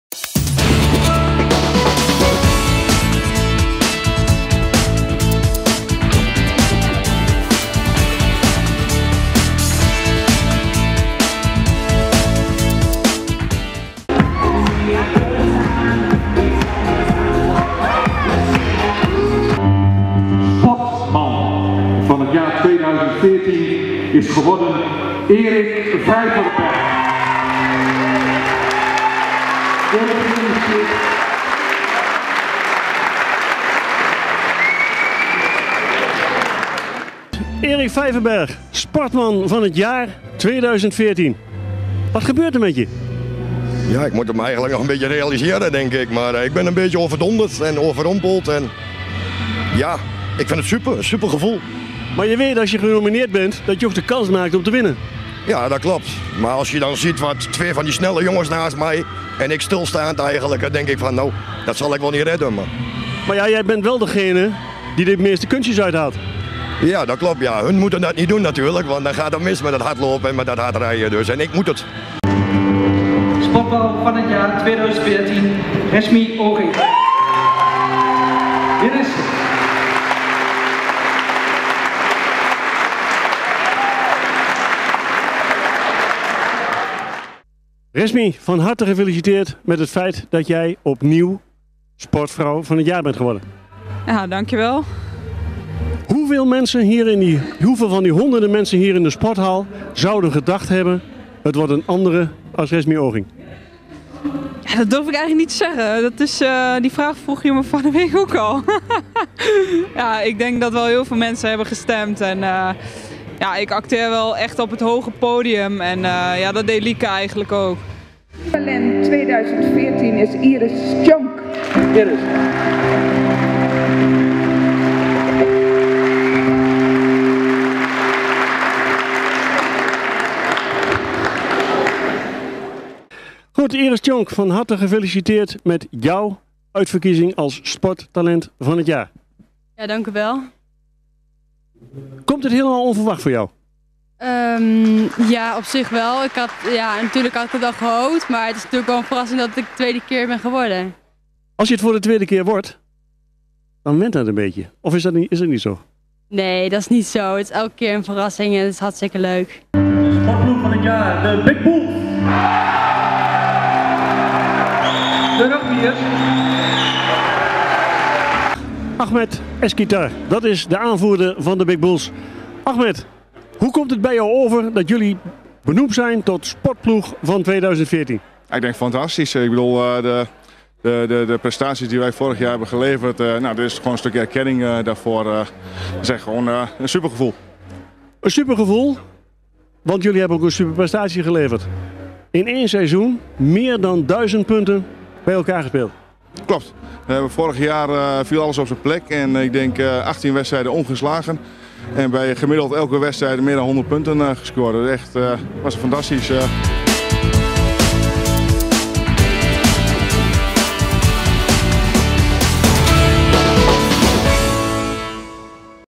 Sportverkiezing 2014 in de IISPA in Almelo
Op vrijdag 30 januari werden de sportman, -vrouw, -talent, -ploeg en sporter/-ploeg met een beperking gekozen. Dit een samenvatting van die avond.